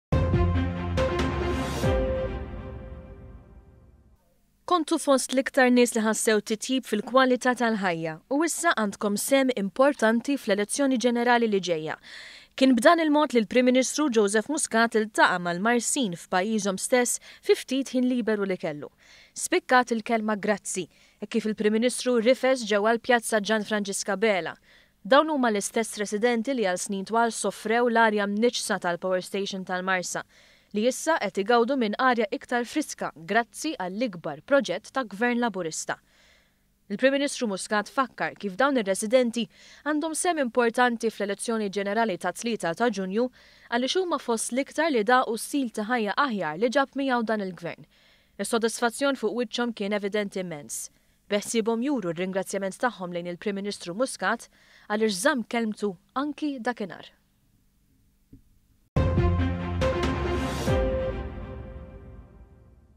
IL-MARSIN JILQGĦU LILL-PRIM MINISTRU.
ONE.COM.MT • Waqt żjara a fil-Marsa li għamel il-Prim Ministru Joseph Muscat, l-iktar frażi li spikkat kienet ‘grazzi ta’ kollox’. Fi kliem il-Prim Minstru stess dawn huma fost l-iktar residenti li daqu stil ta’ ħajja aħjar, fosthom bl-ikbar proġett ta’ Gvern Laburista: l-għeluq tal-powerstation tal-Marsa.